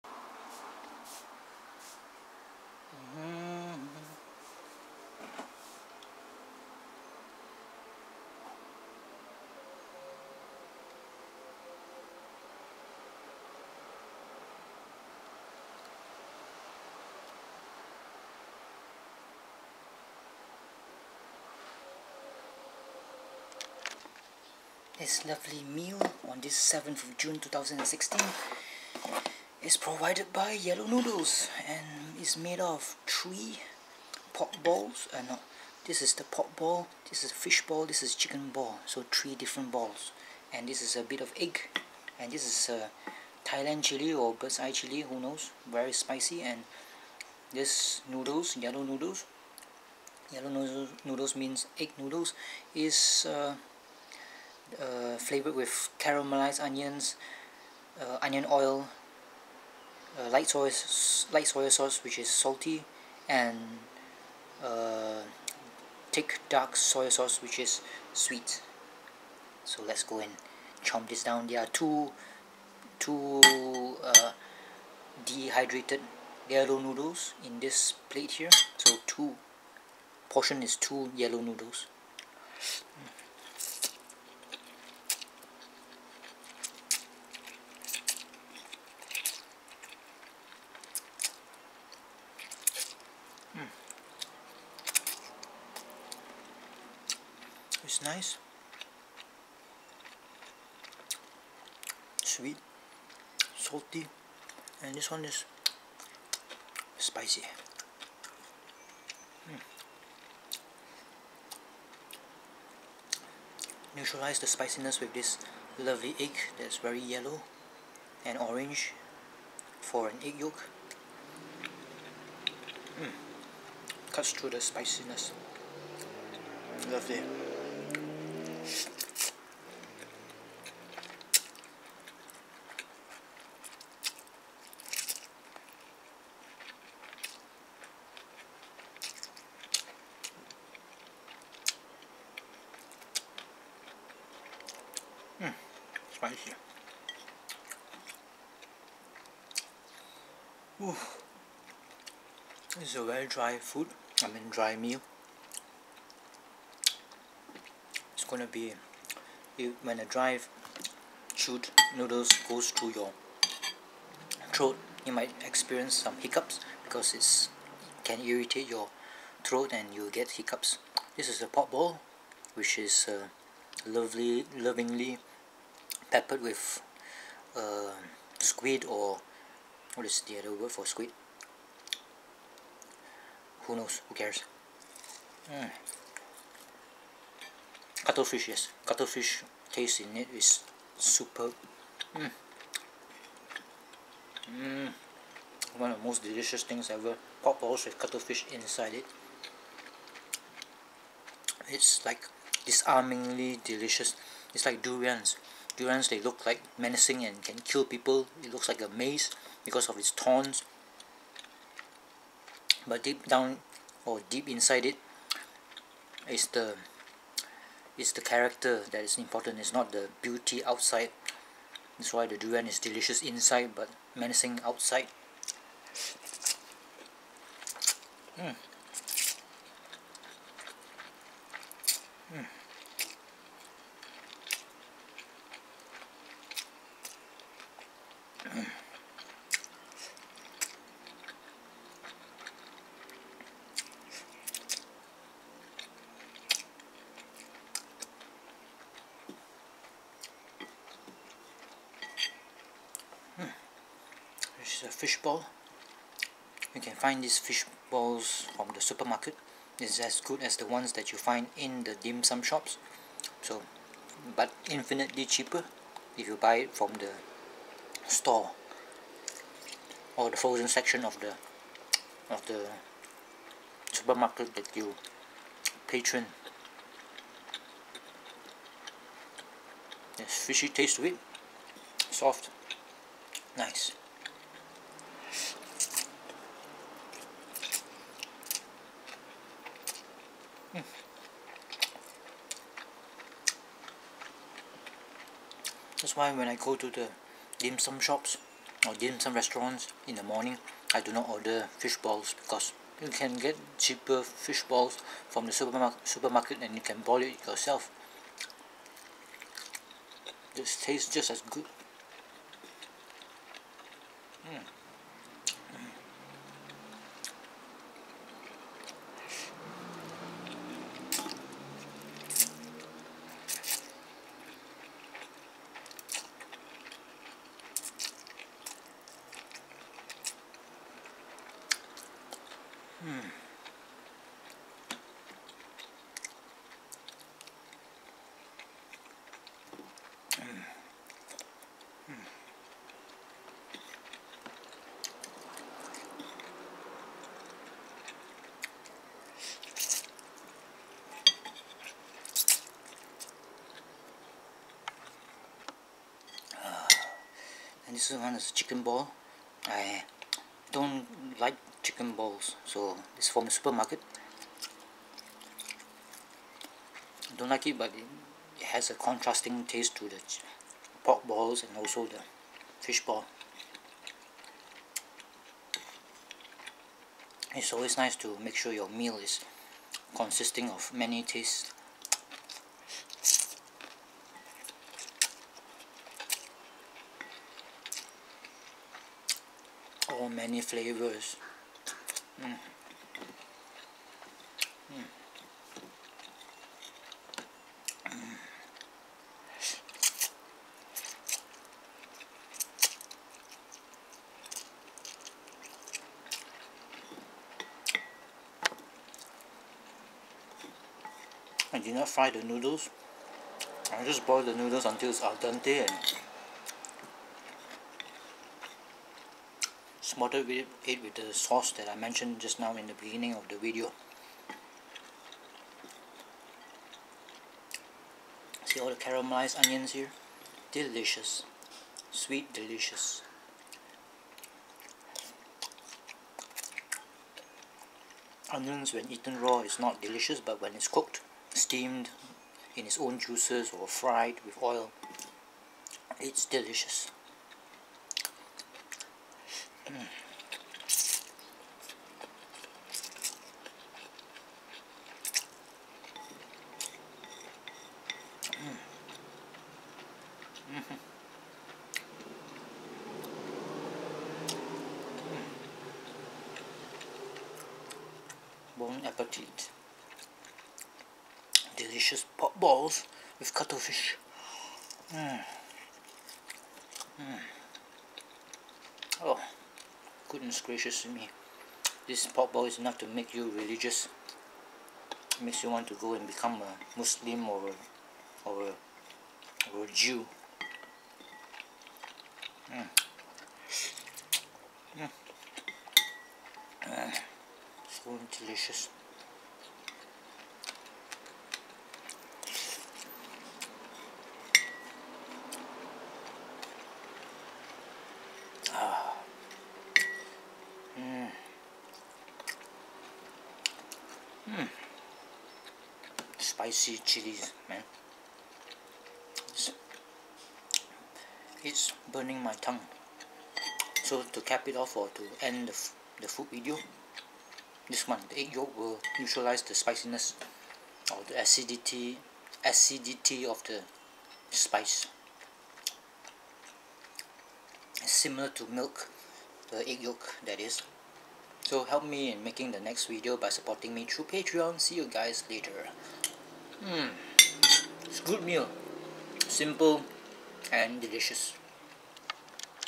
Eating Noodles with 3 Types of Balls, ASMR
Noodles are an essential ingredient and staple in Chinese cuisine. Chinese noodles vary widely according to the region of production, ingredients, shape or width, and manner of preparation. They are an important part of most regional cuisines within China, as well as in Singapore, and other Southeast Asian nations with sizable overseas Chinese populations.

Chinese-style noodles have also entered the cuisines of neighboring East Asian countries such as Korea (jajangmyeon) and Japan (ramen), as well as Southeast Asian countries such as Vietnam (hủ tiếu and mì xào are both examples of Vietnamese dishes that are of Chinese origin), the Philippines, Thailand, and Cambodia.